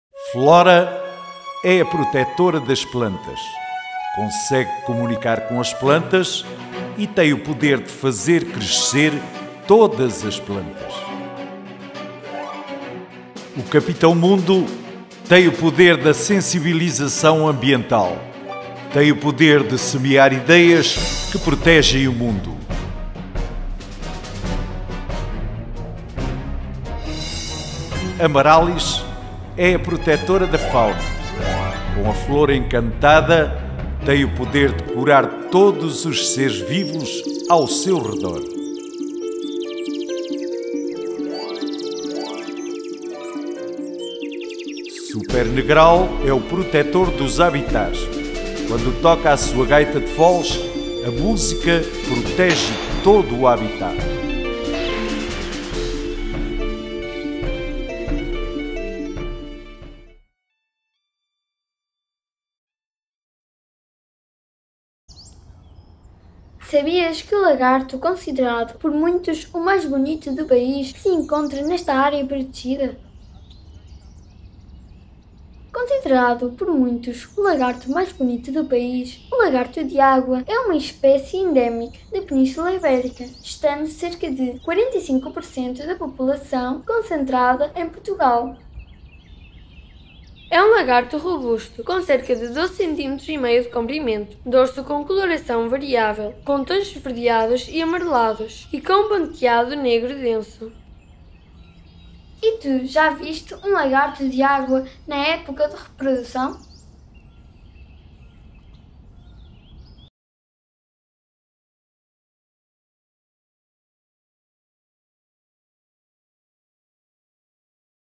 FNT08E17 - PN do Litoral Norte: o lagarto de água
Sabias que o lagarto considerado por muitos o mais bonito do país se encontra nesta área protegida?